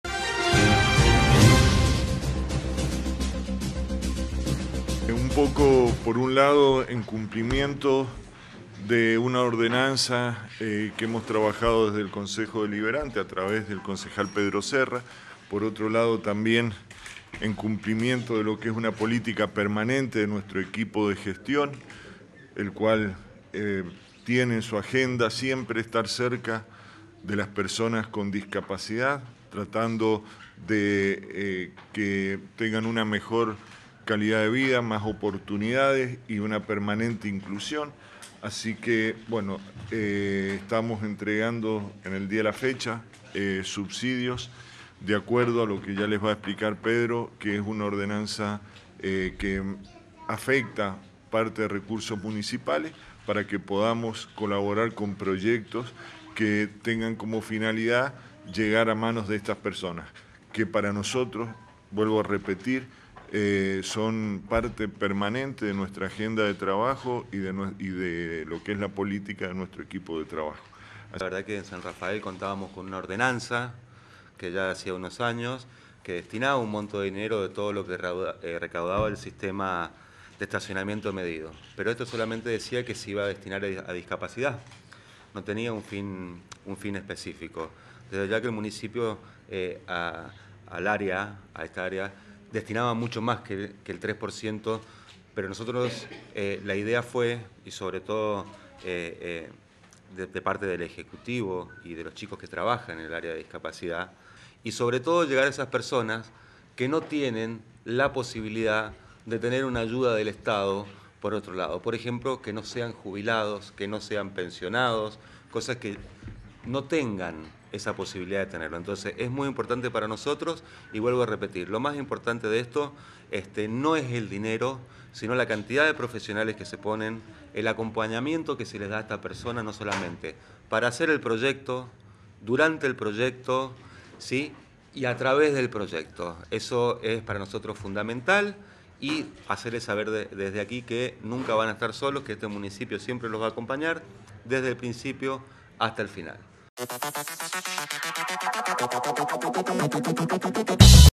DESTACANDO ESFUERZOS
El presidente del HCD local Ricardo Vergara, y su par de banca Pedro Javier Serra, junto al Área de Discapacidad, entregaron este viernes subsidios para emprendedores .